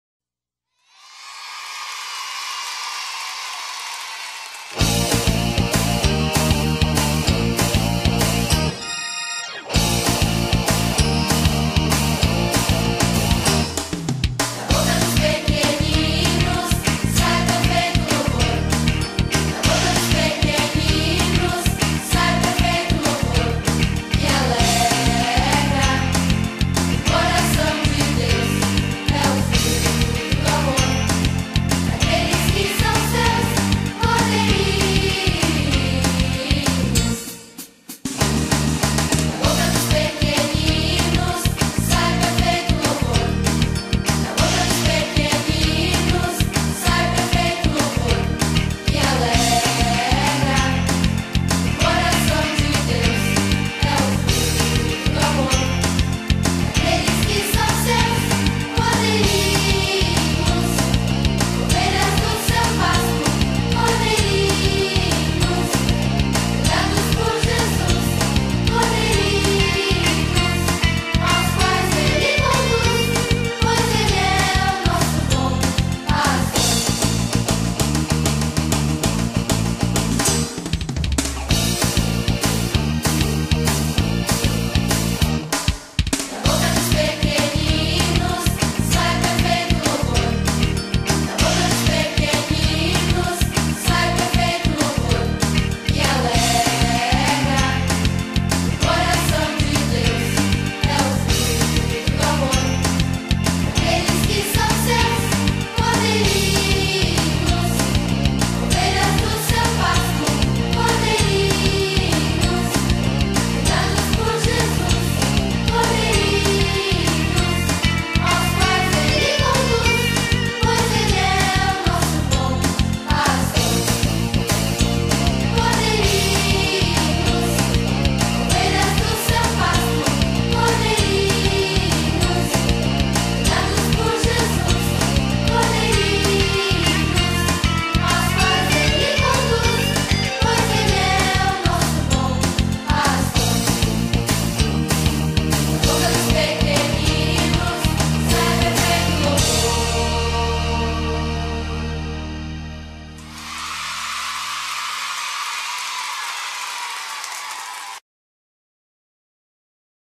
EBF 2009 - Comunidade da Graça São Mateus
Um pouquinho do que foi a EBF 2009 na Comunidade da Graça em São Mateus.